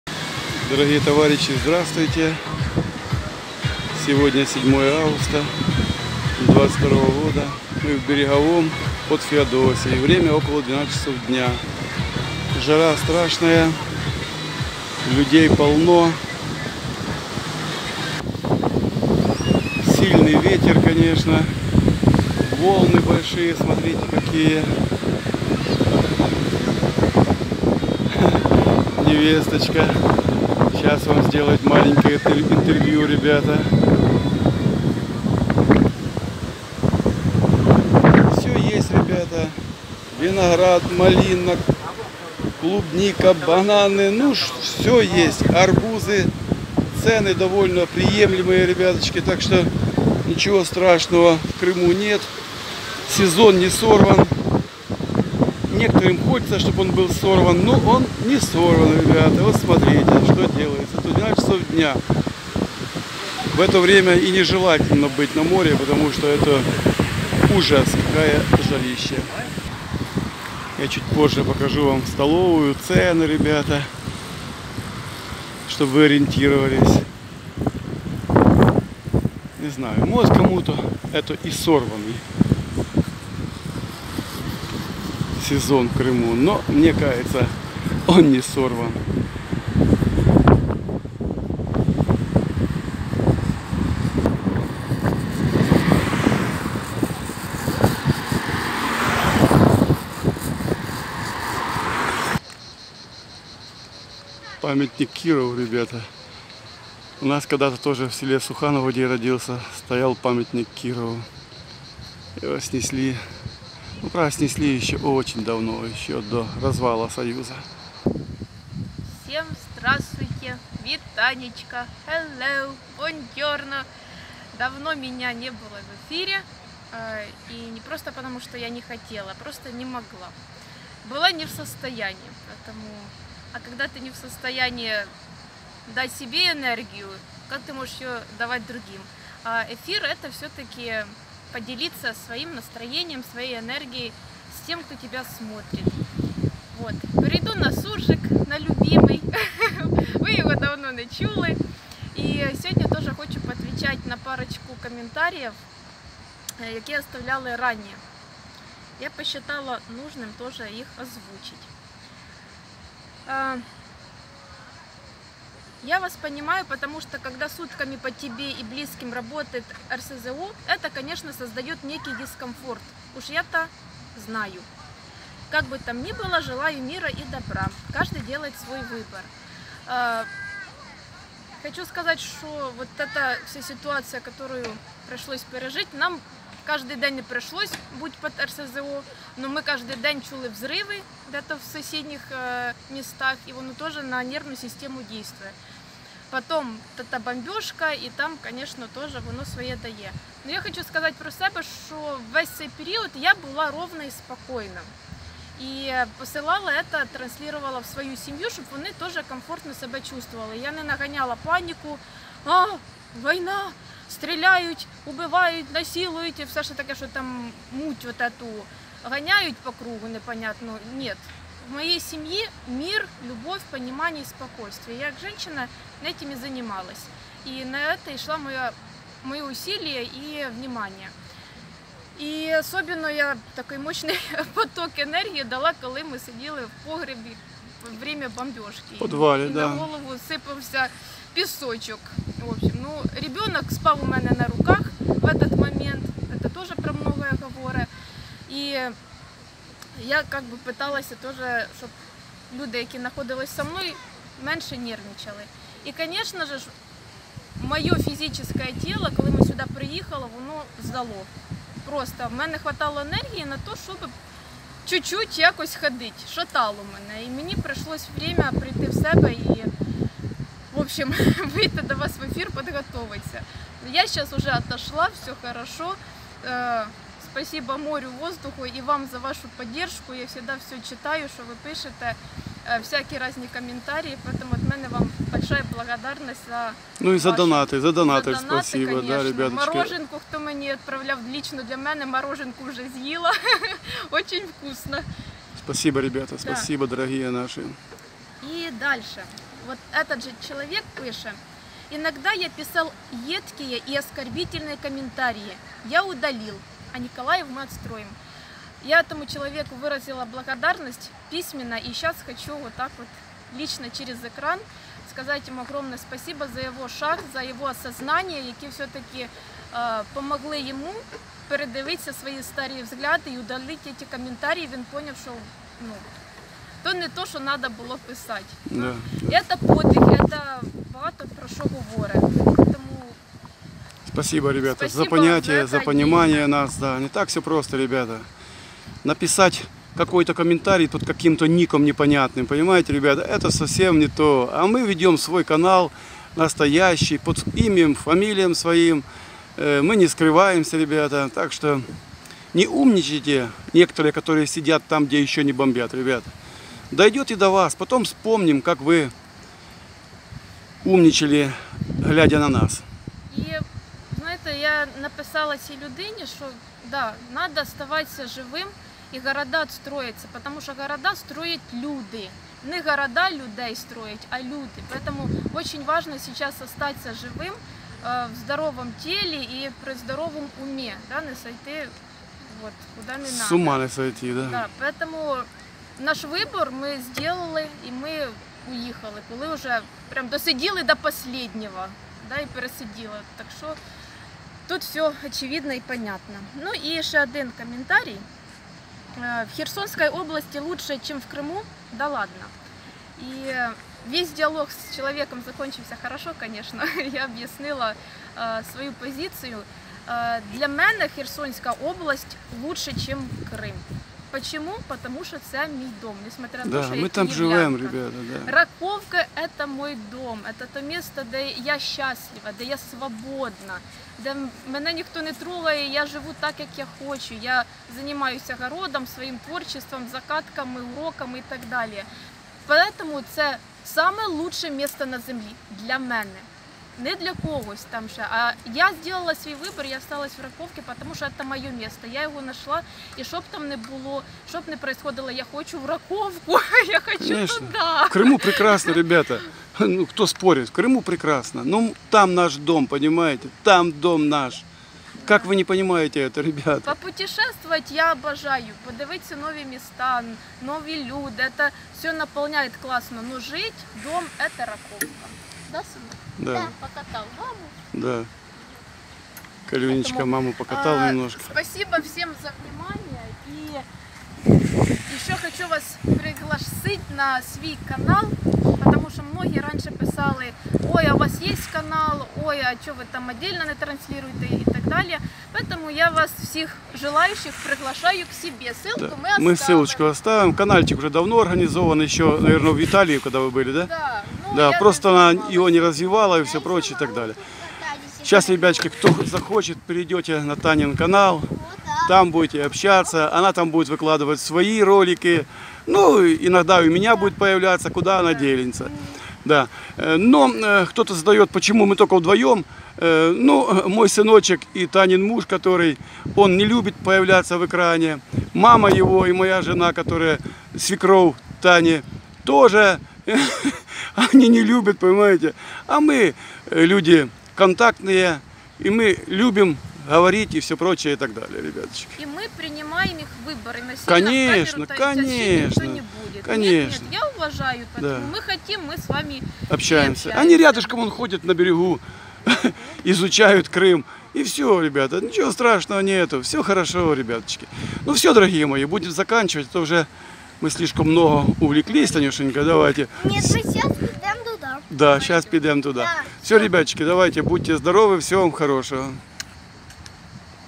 Крым глазами уехавших из Херсонской области...
Говорим и показываем ВСЕ как есть без прикрас !